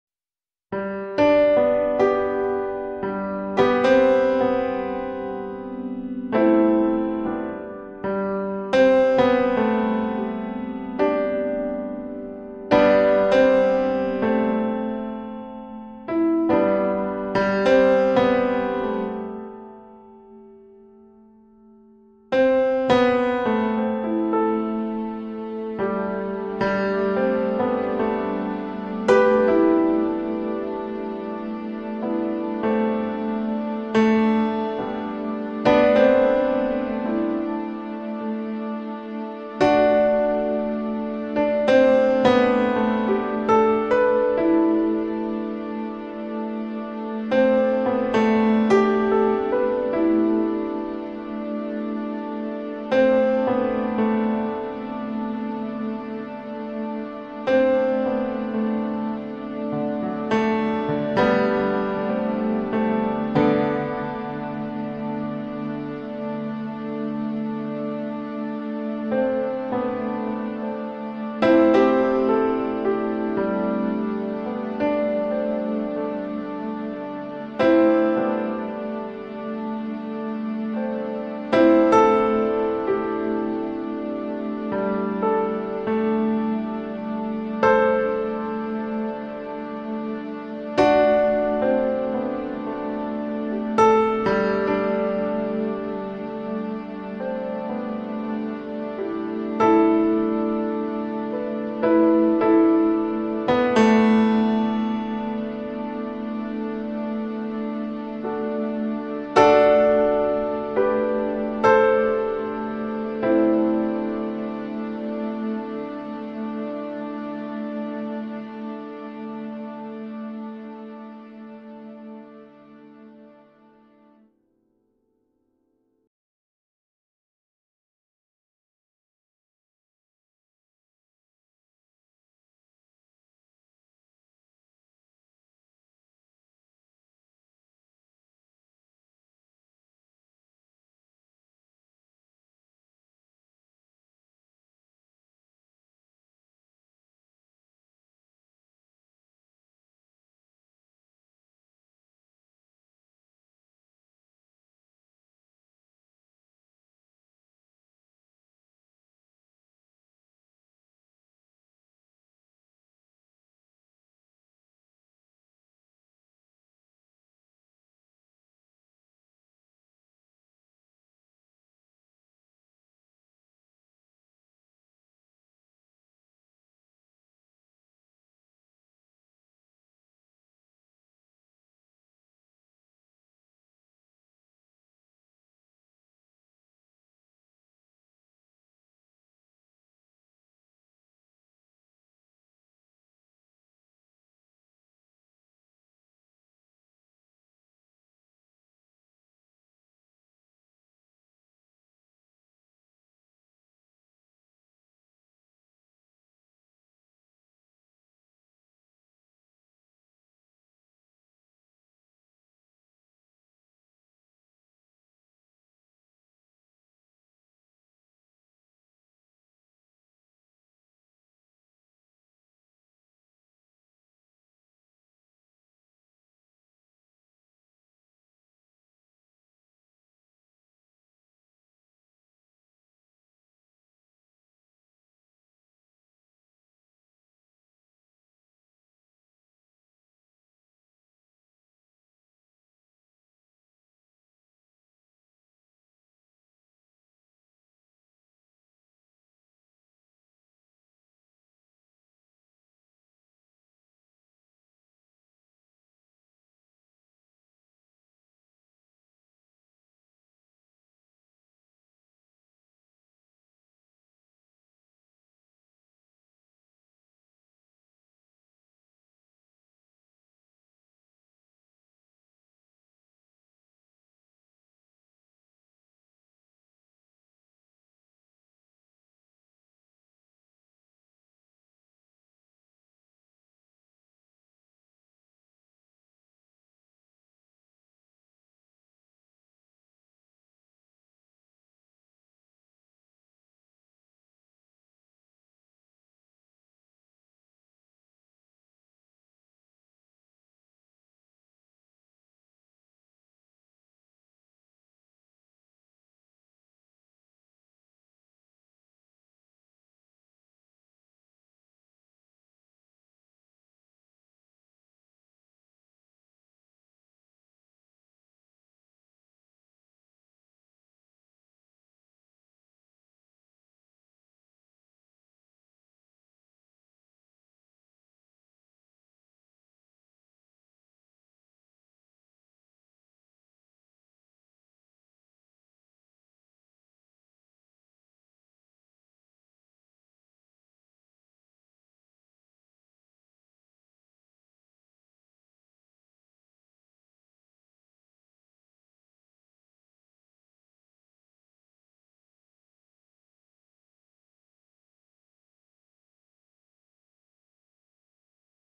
Yangon, A good cat gives birth/Cats Exclusive Birth/Cat giving birth 4 kittens
Kittens Eve Number 4/These videos were recorded from live streaming cams at the Tiny Kittens
Yangon,  cat gives birth